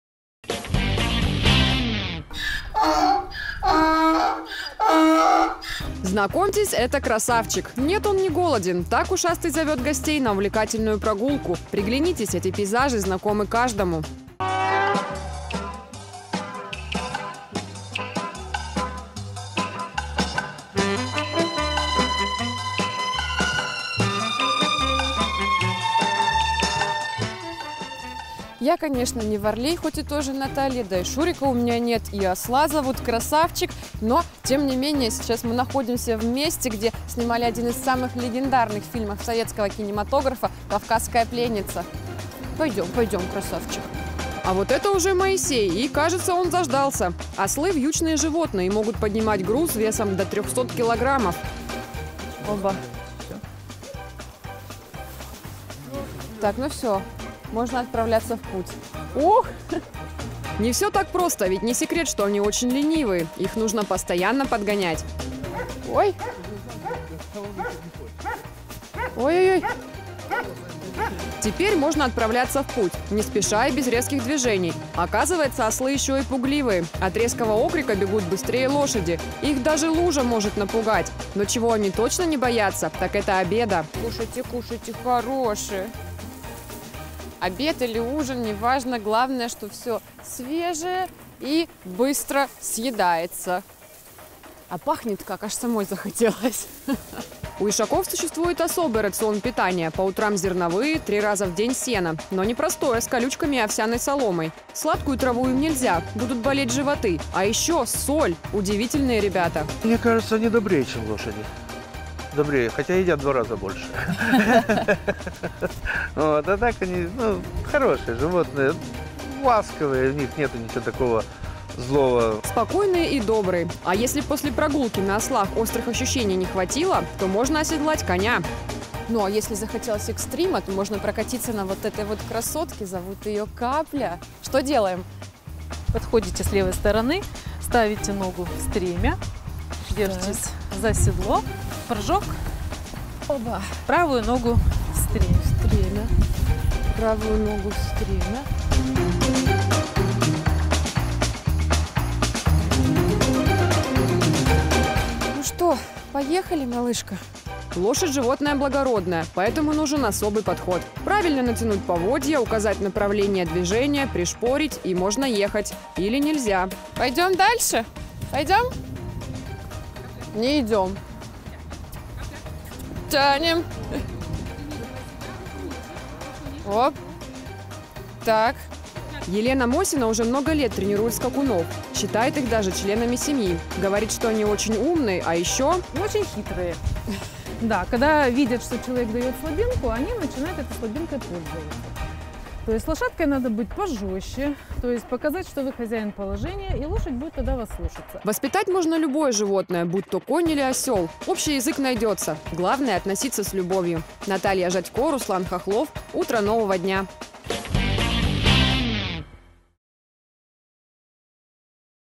Моисей, Красавчик, Крымская пленница, или новые приключения журналистов
У ишаков существует особый рацион питания. По утрам – зерновые, три раза в день сено, но не простое – с колючками и овсяной соломой. Сладкую траву им нельзя – будут болеть животы. А еще – соль! Удивительные ребята.